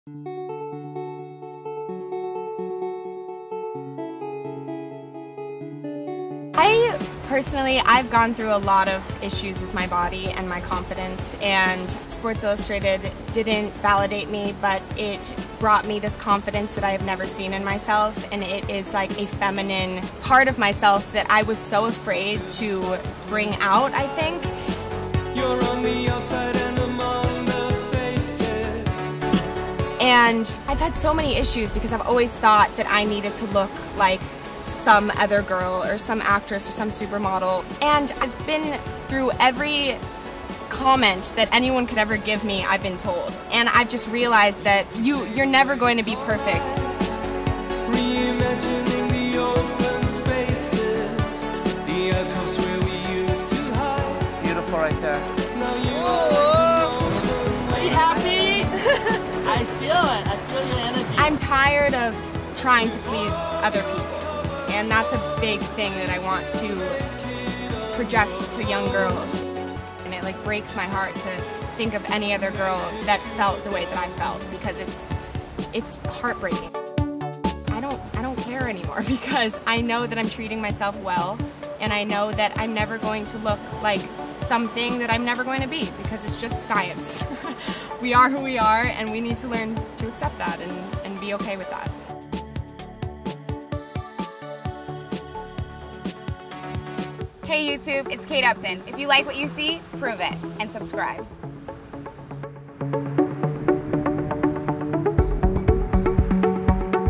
Sailor Brinkley Cook Has an Important Message for YOU | CANDIDS | Sports Illustrated Swimsuit
Sailor Brinkley Cook reveals her struggles and difficulties she has faced throughout the years. She also has an important message to all young people on accepting and making the best out of what you have.

Sailor Brinkley Cook Has an Important Message for YOU